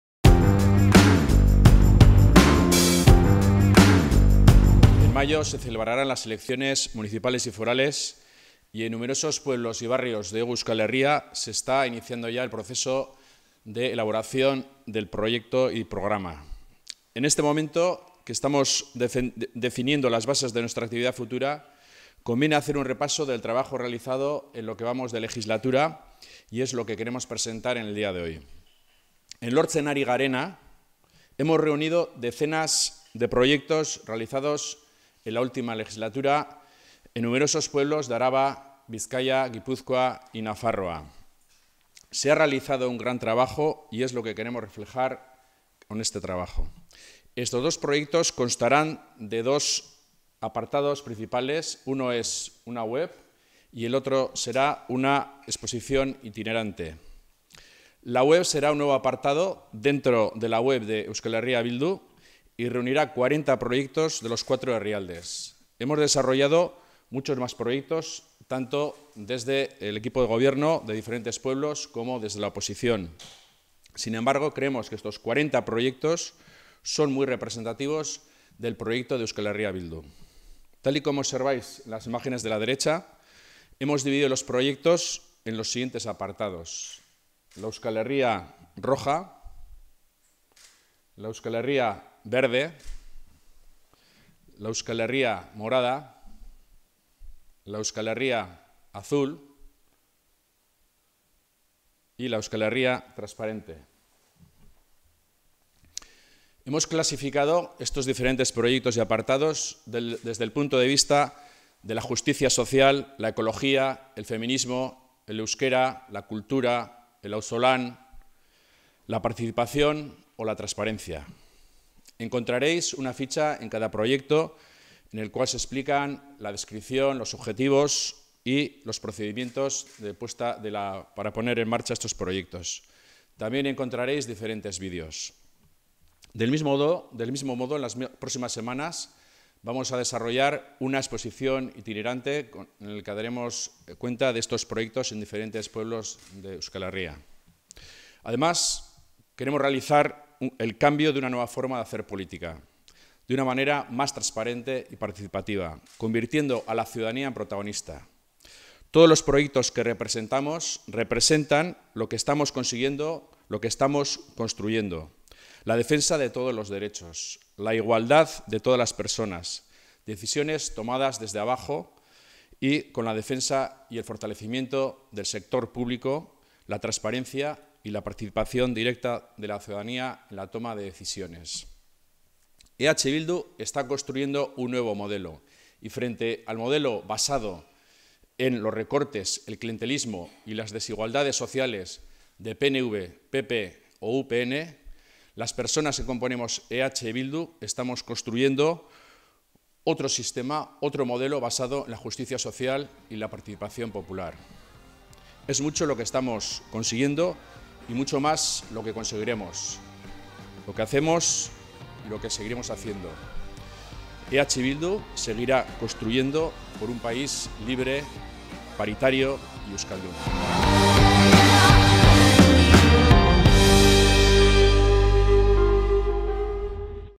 Hemos presentado la web "Tejiendo el futuro"
Frente al modelo basado en los recortes, el clientelismo y la desigualdad social de PNV, UPN y PP, EH Bildu está construyendo un modelo basado en la justicia social y la participación popular.